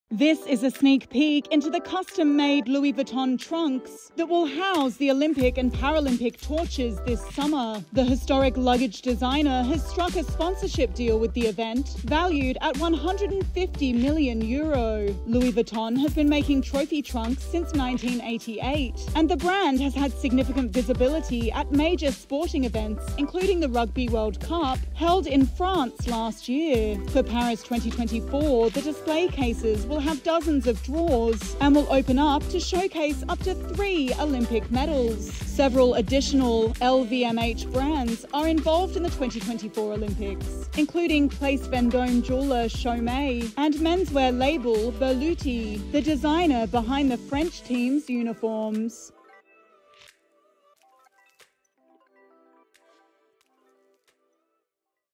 Sneak Peak Into Luxury Designed Louis Vuitton Torch and Medal Trunks for Paris 2024
This is a sneak peek into the custom-made Louis Vuitton trunks that will house the Olympic and Paralympic torches this summer. Buzz60's Chloe Hurst has the story!